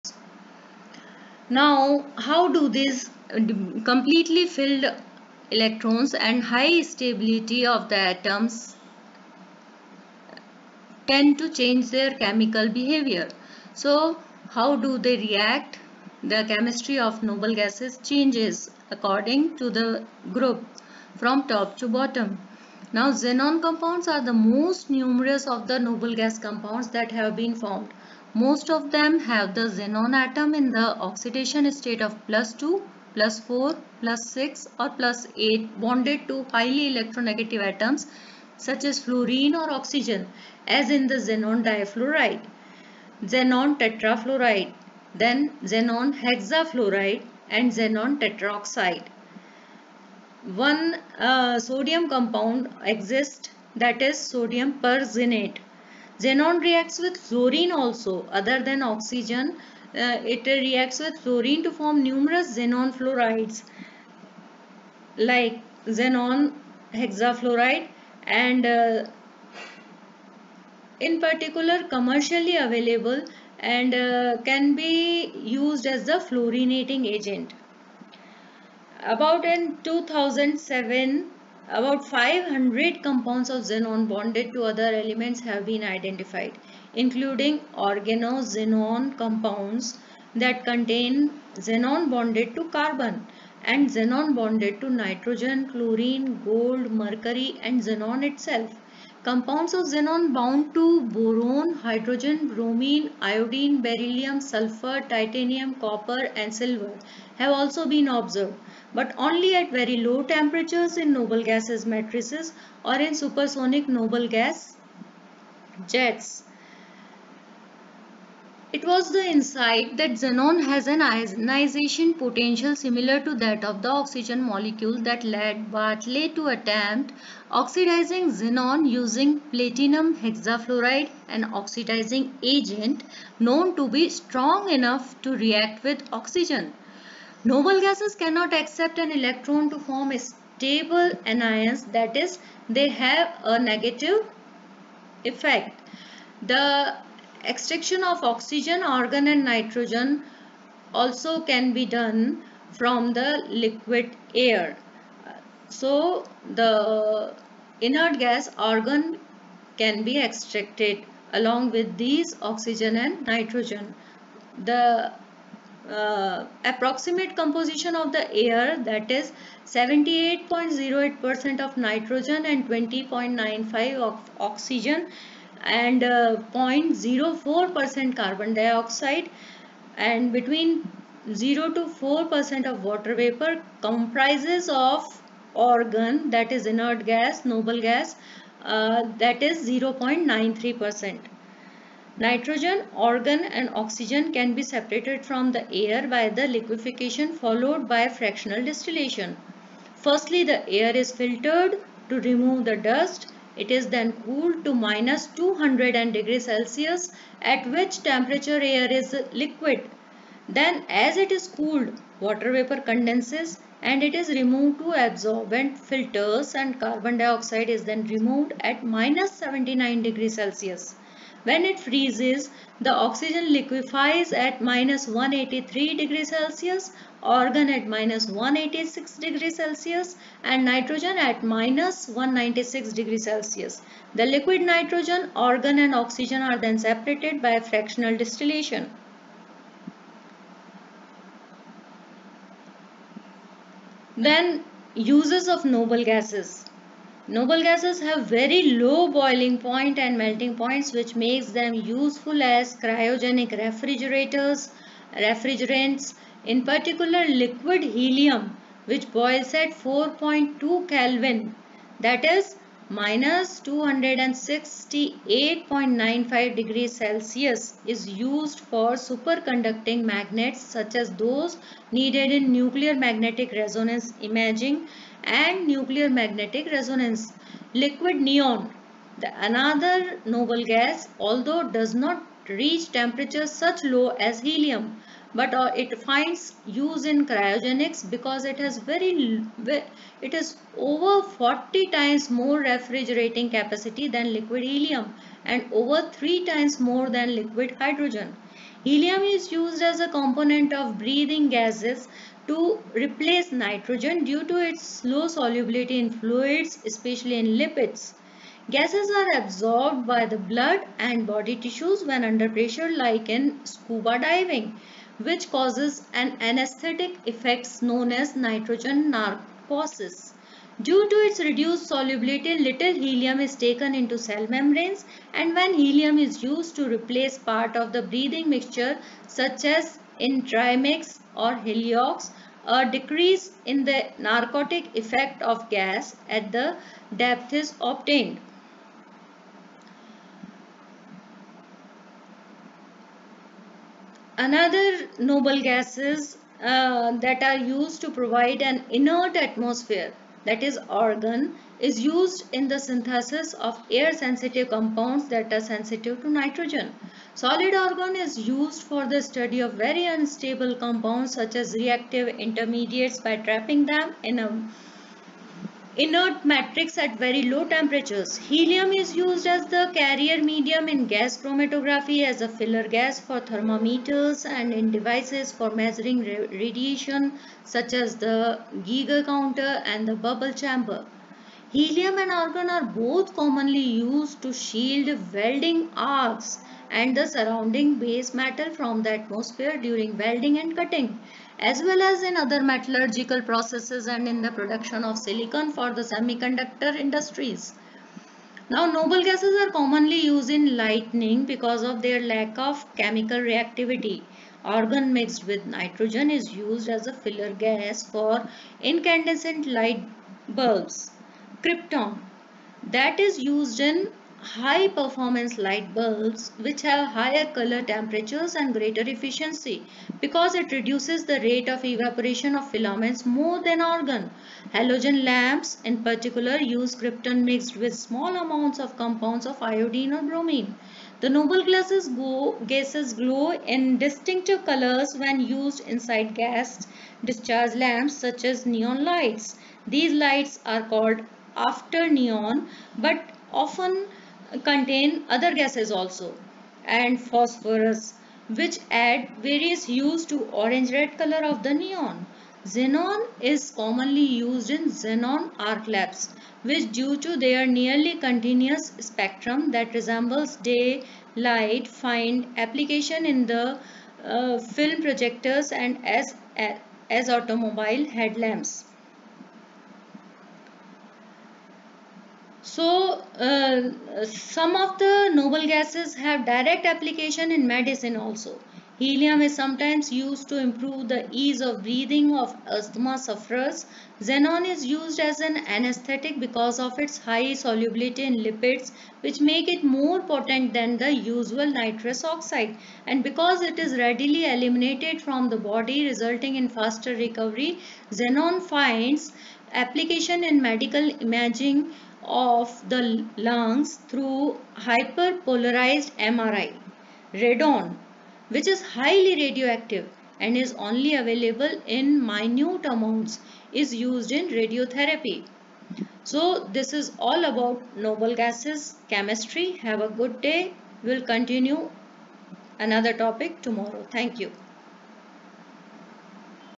Chemistry of noble gases part 2 by Dr. Rekha Bhatt
Separation, extraction, uses, and applications of noble gases.